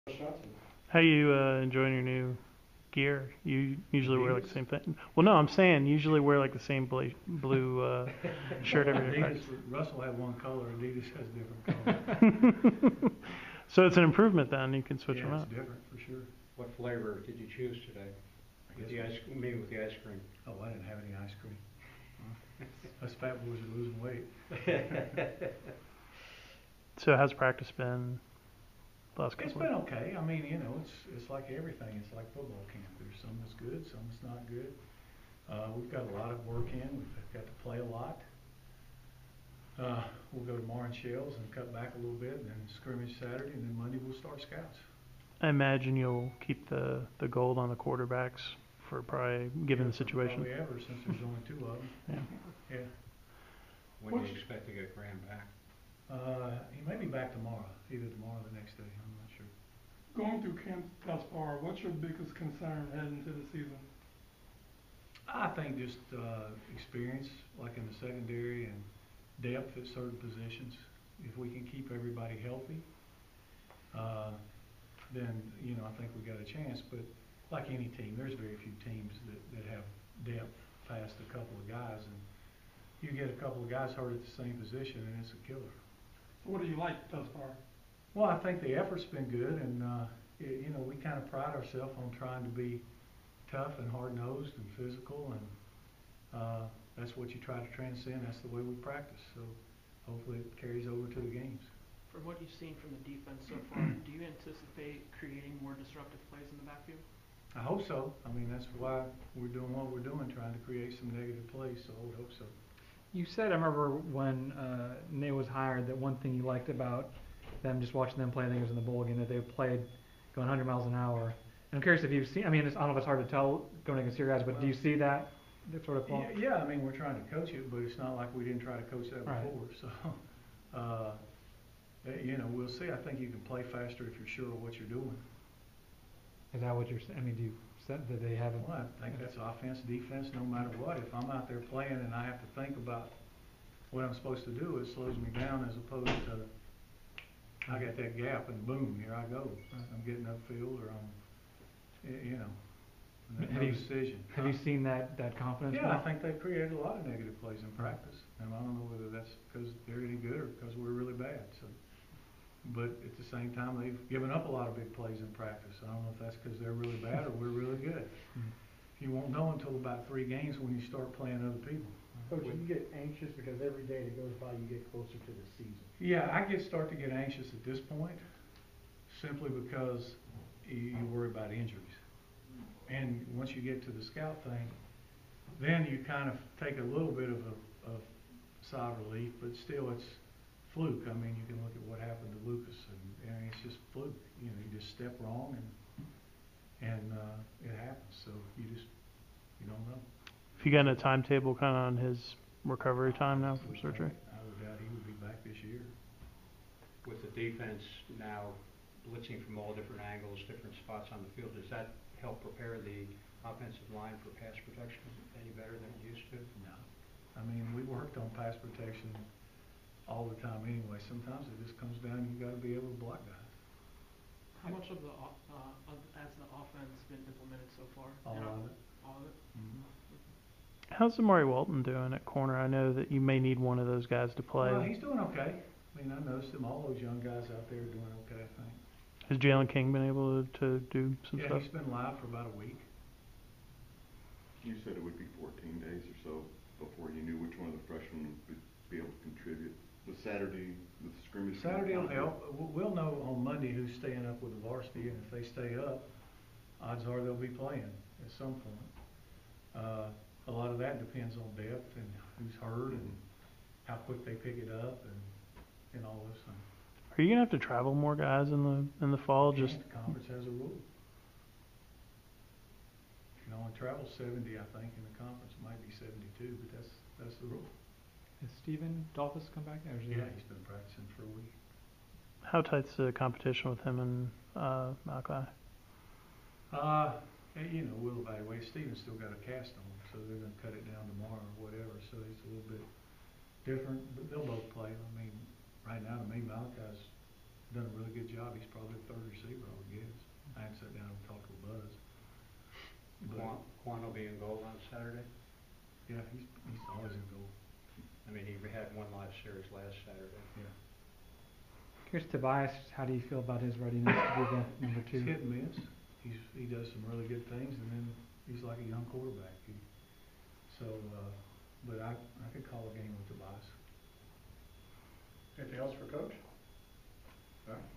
JOL TV: Paul Johnson 8/16
Georgia Tech football coach Paul Johnson talks about the effort in camp and how he sees some position battles unfolding over the last few days of Fall Camp.